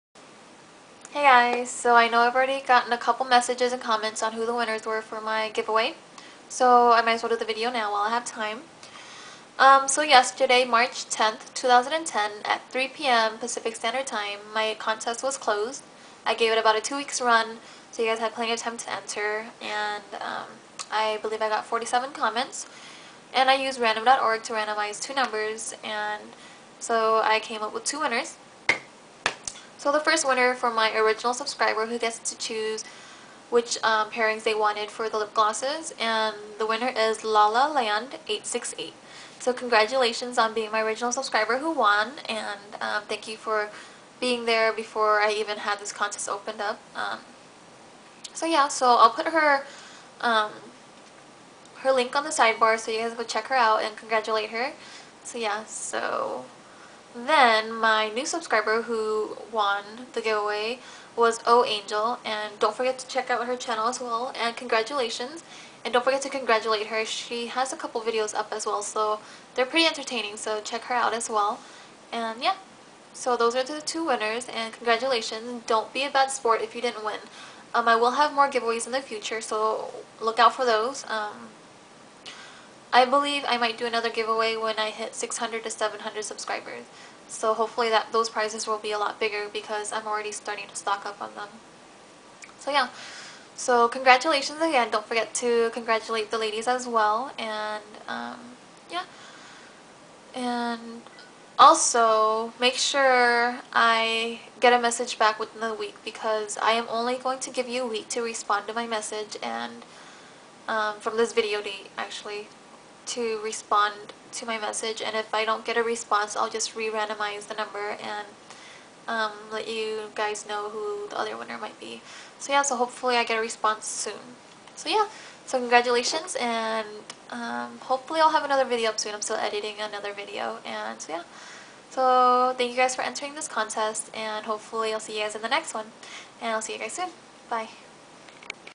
Giveaway Winner!!
Thank you all for entering this giveaway, Don't be upset if you didn't win this one; there are more to come in the future!

Don't forget to congratulate these ladies and I'll see you guys again soon!

chauberry